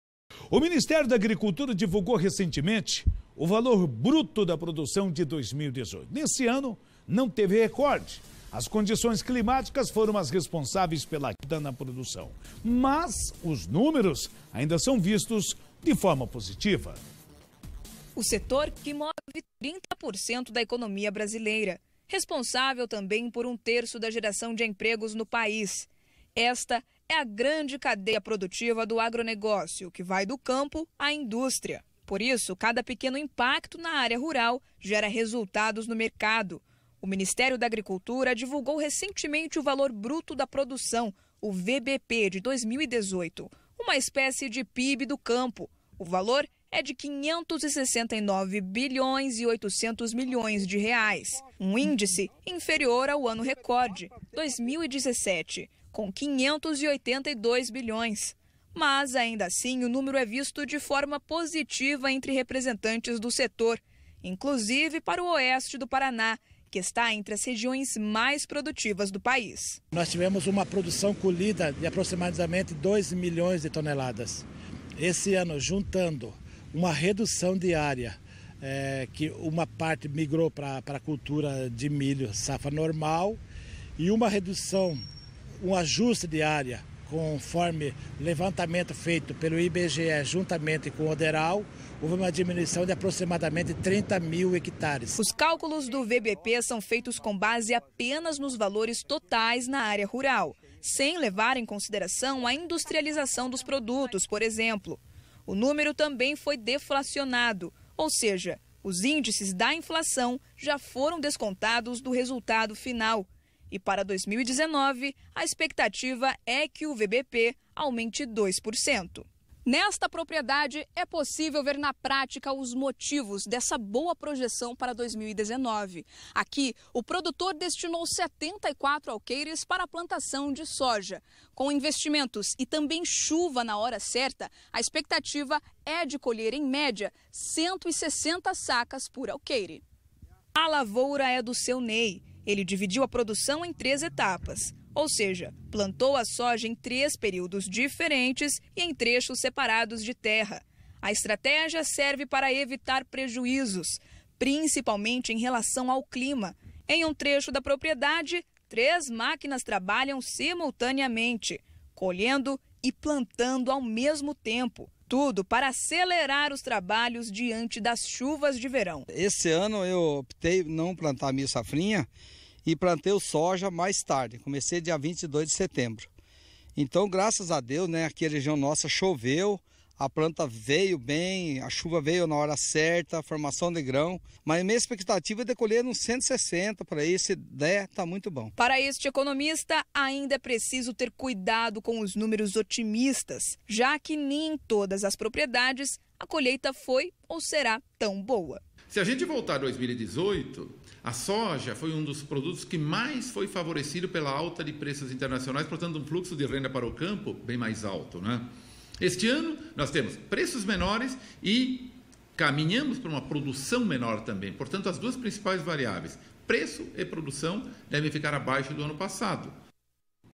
Apesar das adversidades climáticas o valor bruto de produção ainda é positivo
O Ministério da Agricultura recentemente divulgou o valor bruto da produção de 2018 e é claramente perceptível que condições climáticas adversas tem culpa na queda de produção, porém, os números não são exatamente negativos e podem ser olhados de forma otimista. Acompanhe.